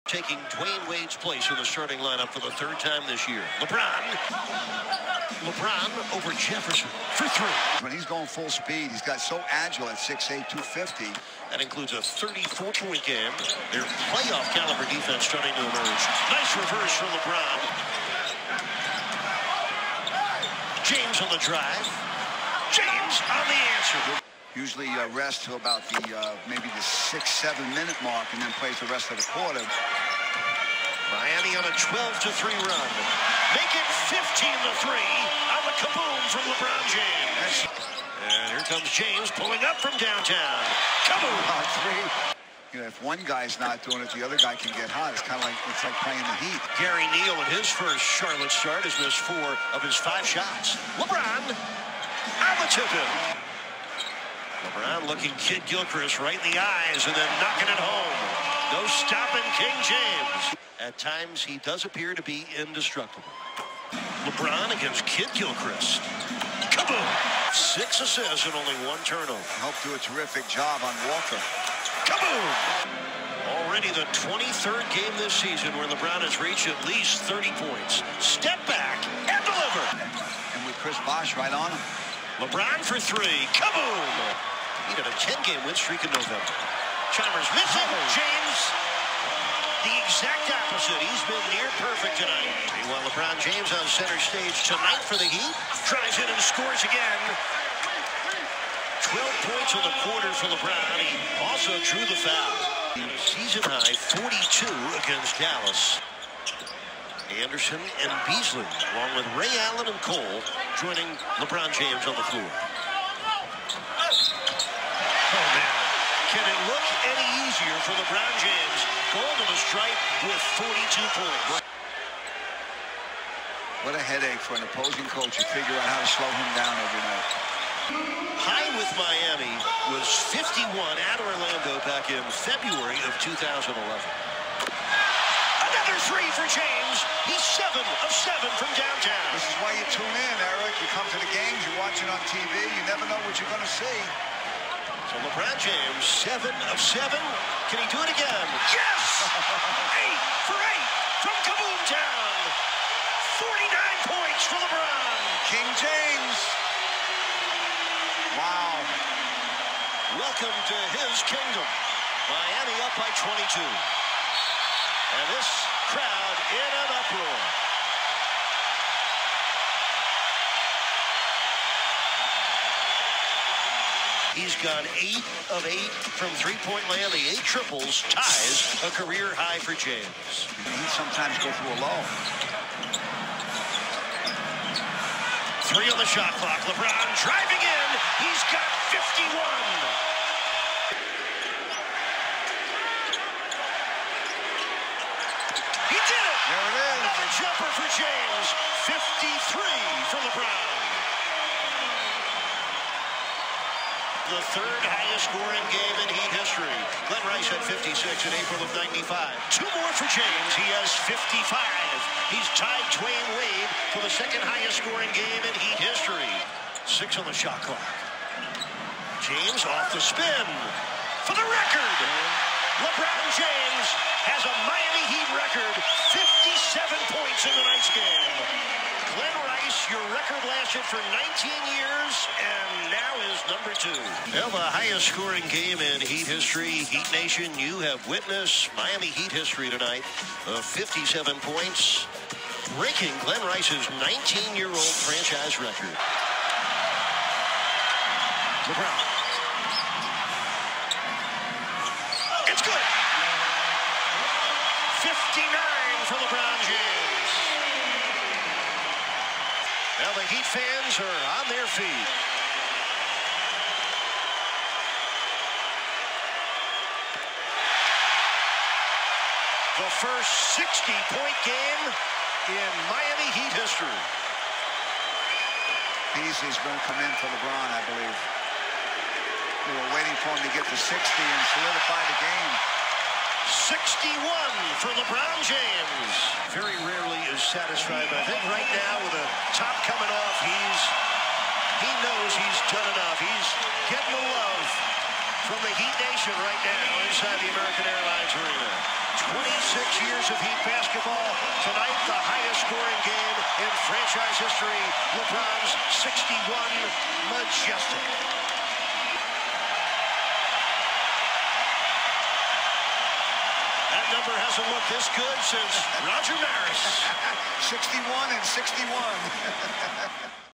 LeBron James' career high 61 points sets Miami Heat franchise record | March 3, 2014 vs. Bobcats
3/3/14: LeBron James puts up a career high 61 points to set a new Miami Heat record for most points scored in a game previously held at 56 points by Glen Rice in 1995